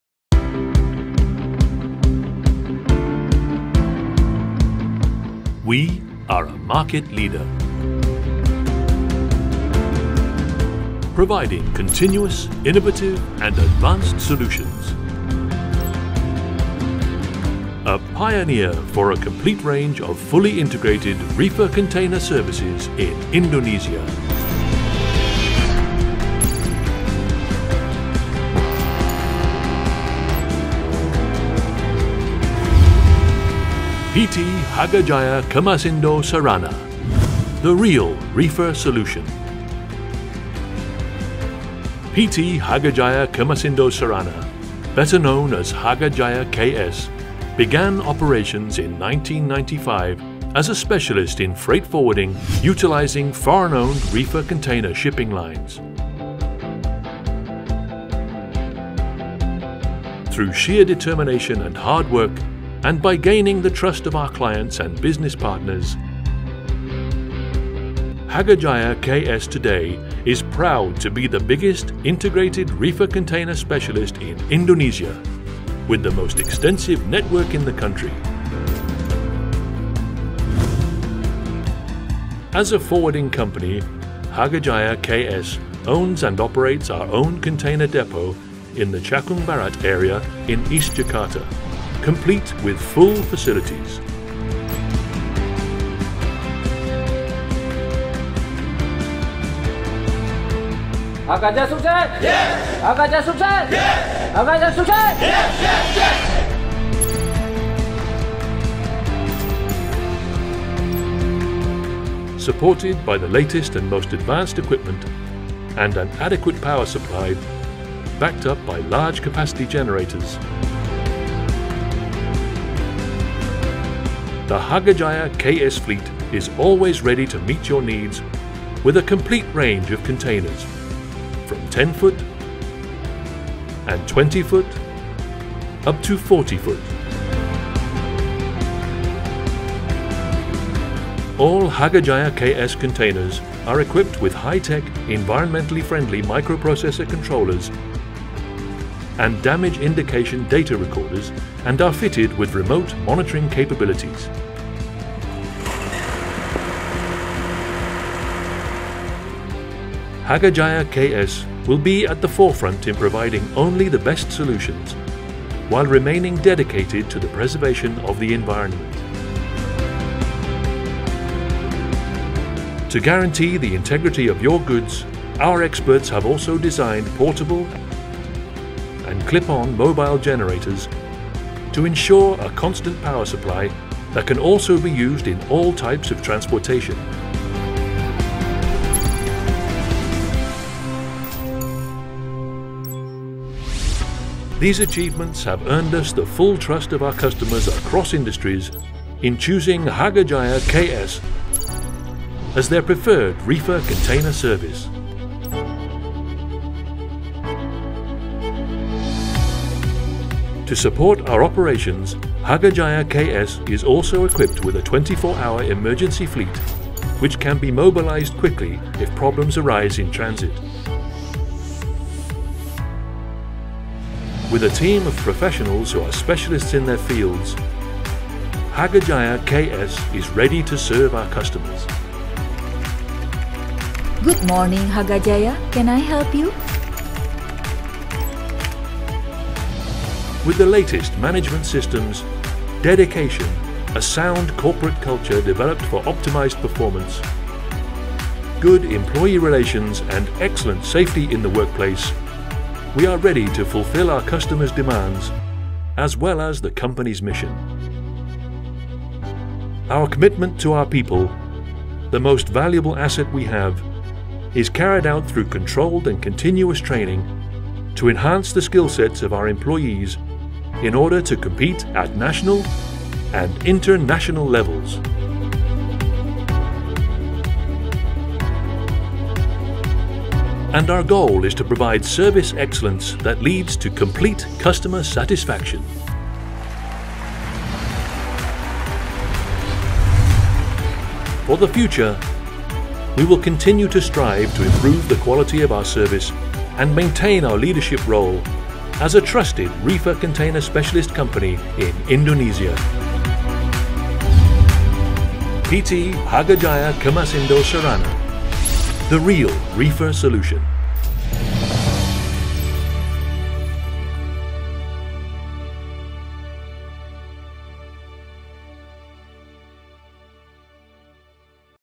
Company Profile Video Hagajaya Ks®️ (English version)
Video Company Profile of PT Hagajaya Kemasindo Sarana ( Hagajaya KS ® )

A freight forwarding company in Indonesia, founded in 1995 in Jakarta, under the leadership of Mr. Handoko Supranoto and his teamwork. A forwarding company that specializes in the transportation of both frozen and chiller (Perishable Goods) cargo for domestic links from and to all major ports in Indonesia (Inter Island and land transportation). Companies based in Jakarta, with their own container depot land, and equipped with units. - Owned refrigerated container unit (reefer), complete generator units, and equipped with a sufficient trucking fleet. Also provide reefer units and generators that are ready to rent for static storage and mobile transport. For more details, you can watch this video profile and also visit the official